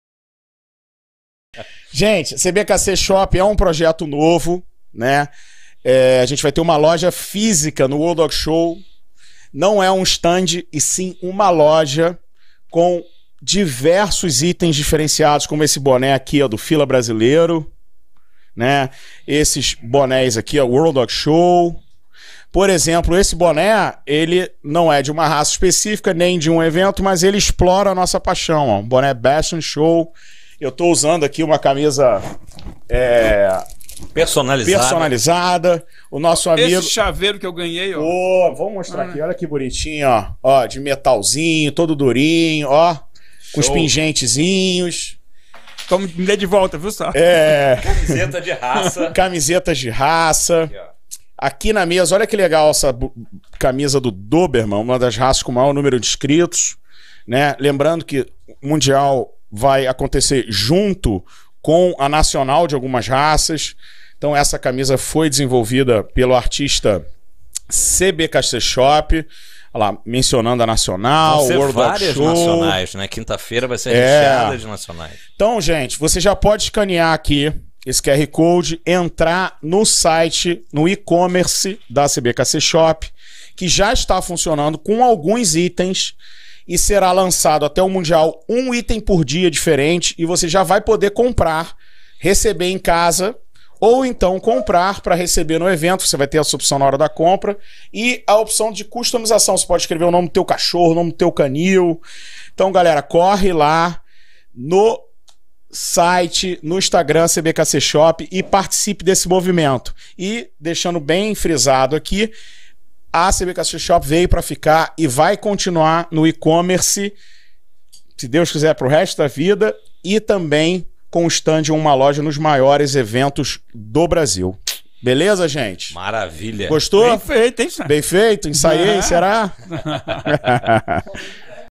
FAQ - CBKC SHOP
Os produtos oficiais do World Dog Show Brasil 2022 estarão disponíveis apenas na CBKC Shop. Clique e descubra mais. Para comprar ou reservar, acesse

The official products of the World Dog Show Brazil 2022 will only be available at the CBKC Shop. Click to find out more.